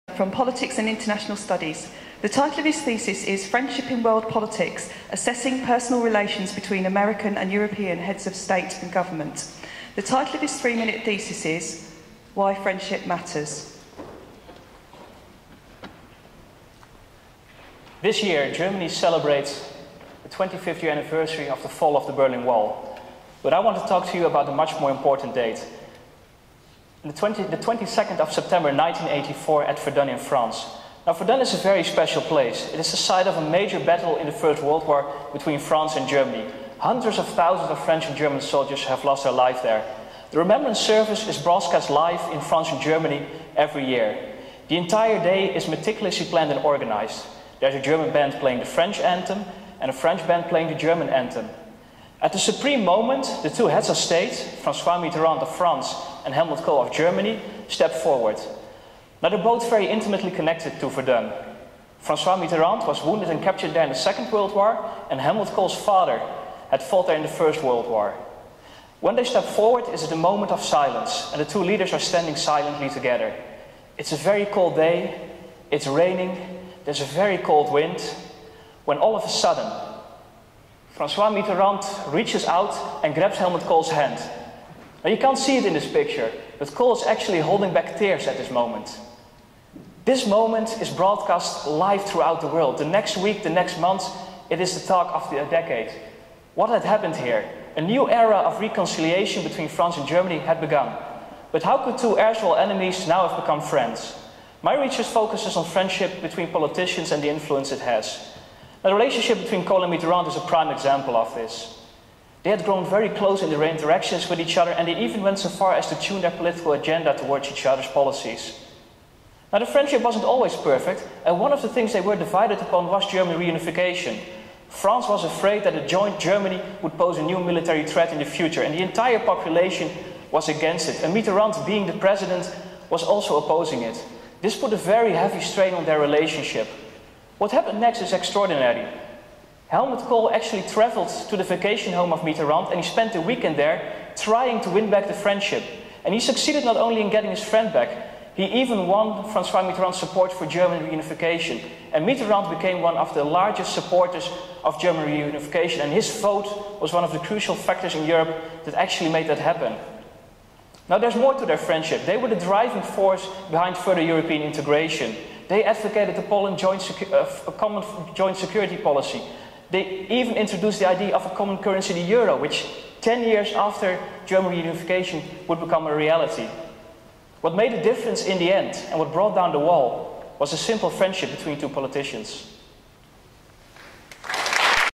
Yuri van Hoef 3MT University of Leeds 2014
Thesis title: Friendship in World Politics. Assessing personal relations between American and European heads of state and government​
Three Minute Thesis title: Why friendship matters